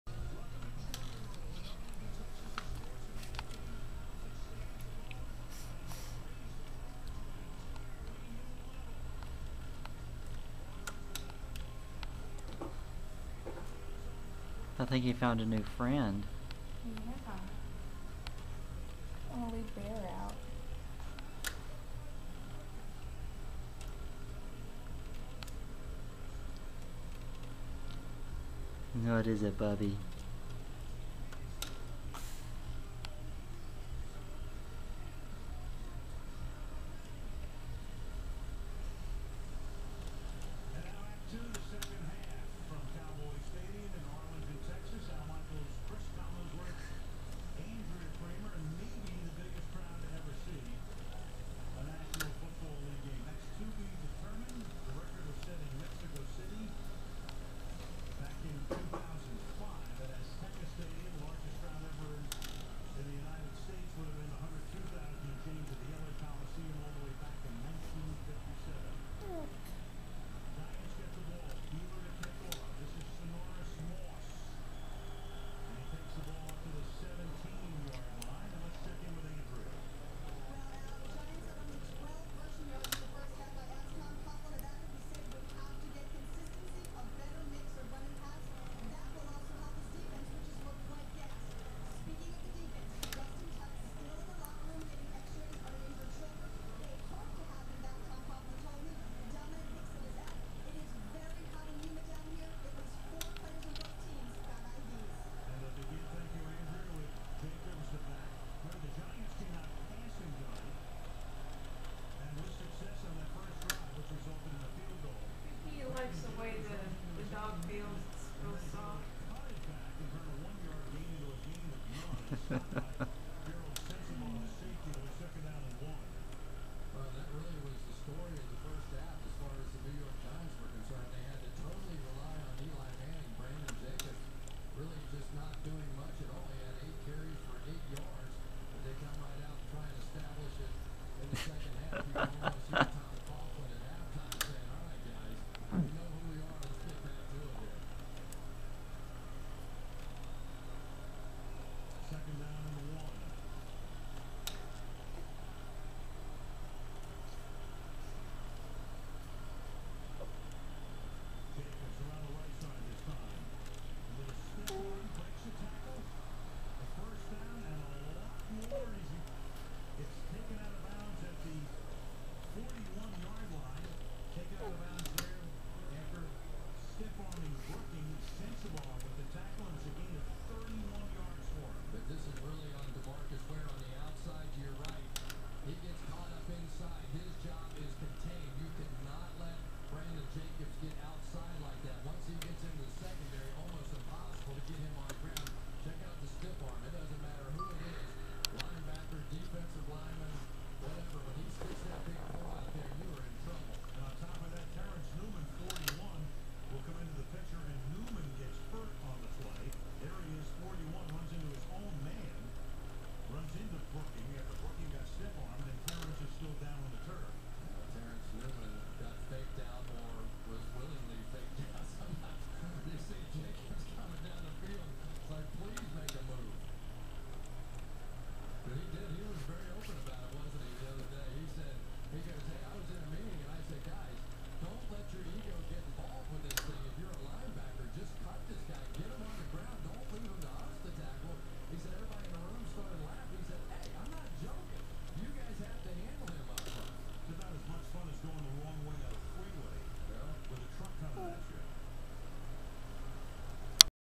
Baby James playing with stuffed animals & is kinda sleepy (2 1/2 months old)
James playing with stuffed animals & is kinda sleepy (2 1/2 months old)